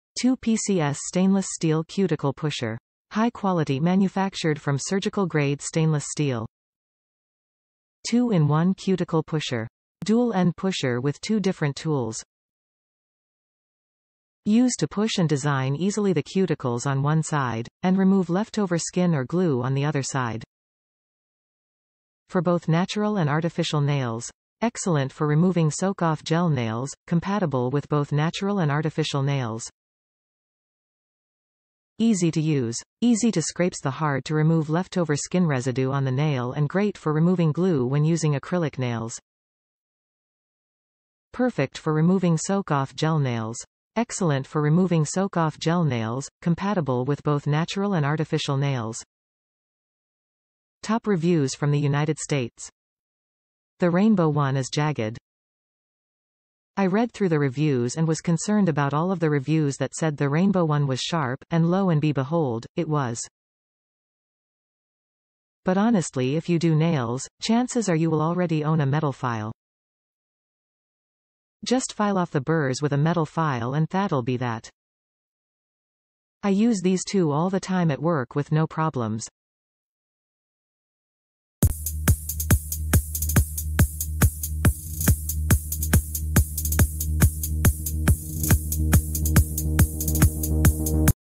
FULINJOY 2PCS Cuticle Pusher and Cutter, Stainless Steel Dual Head Cutter and Trimmer Manicure
【2PCS Stainless Steel Cuticle Pusher】 High quality manufactured from surgical grade stainless steel.
【Two in One Cuticle Pusher】 Dual-end pusher with 2 different tools. Use to push and design easily the cuticles on one side, and remove leftover skin or glue on the other side.
【For Both Natural and Artificial Nails】 Excellent for removing soak off gel nails / Compatible with Both Natural and Artificial Nails.
【Easy to Use】 Easy to scrapes the hard-to-remove leftover skin residue on the nail and great for removing glue when using acrylic nails.
【Perfect for Removing Soak Off Gel Nails】 Excellent for removing soak off gel nails / Compatible with Both Natural and Artificial Nails.
The rainbow one IS jagged
I read through the reviews and was concerned about all of the reviews that said the rainbow one was sharp, and low and be behold, it was. But honestly if you do nails, chances are you will already own a metal file. Just file off the burrs with a metal file and that'll be that. I use these two all the time at work with no problems!